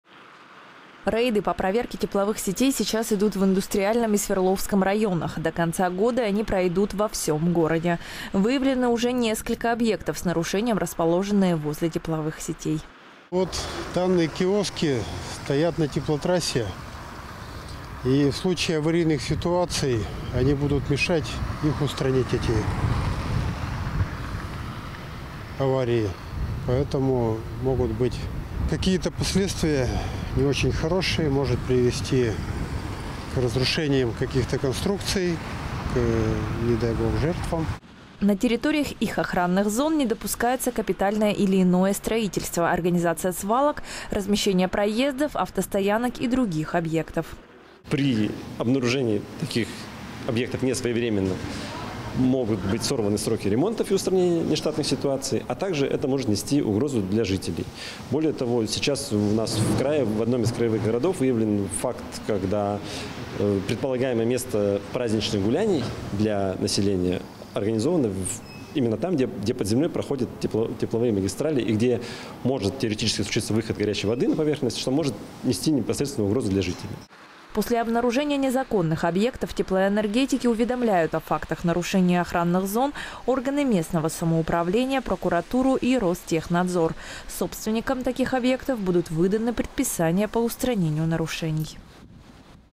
РБК+ БИЗНЕС. Проверка теплосетей.
На территориях охранных зон не допускается капитальное и иное строительство

Эксперты:
Константин Перевозчиков, Cтарший мастер участка тепловых сетей ГЭ №2 ООО «Пермская сетевая компания»
Михаил Топильский, Заместитель руководителя РЦ СК ПАО «Т Плюс» по г. Перми.